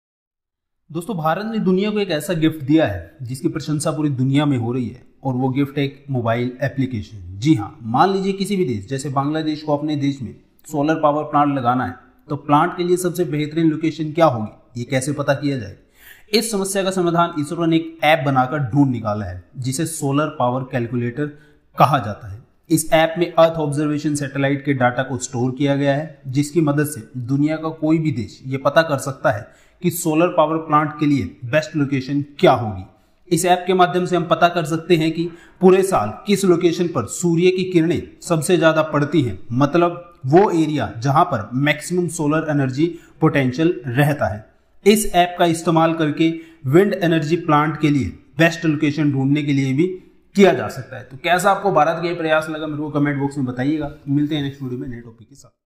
India 🇮🇳 ने दिया दुनियां को तोहफा 🎁
ISRO's Solar Power Calculator
Indian Space Research Organization (ISRO) has recently demonstrated the method for the identification of the best-suited locations for the installation of solar power generation farms across the world. ISRO has achieved this feat using the data it obtained from its own earth observation satellites that continuously monitor the planet from Geostationary Orbit
Dr K Sivan, the Chairman of this ISRO said that Solar Power Calculator was a technology demonstration proposal that was made using data from Earth Observation Satellites. He told a leading daily, "It is about using satellite data and identifying potential areas where maximum solar energy is available per year, across the globe. We will be collecting this data, analysing, mapping it and sharing it online and via a mobile application. Mapping this data and sharing it would help countries decide their upcoming solar power projects."